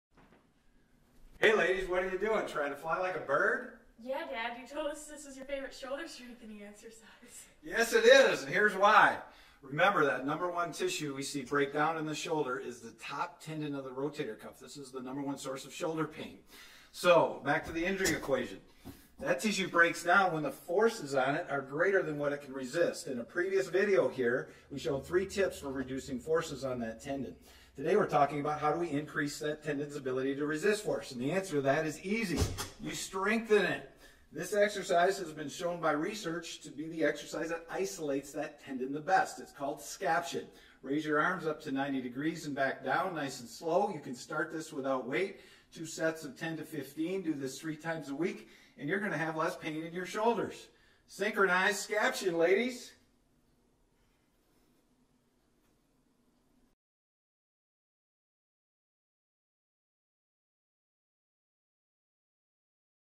BEST SHOULDER STRENGTHENING EXERCISE
Try this simple and effective way to strengthen your shoulders! The top top tendon of the rotator cuff is the number one source of shoulder pain.

Injury equation: Tissue breaks down when the forces on it are greater than what it can resist. This is true for shoulder pain. Try is exercise for shoulder pain from Tom Hulst with Hulst Jepsen Physical Therapy.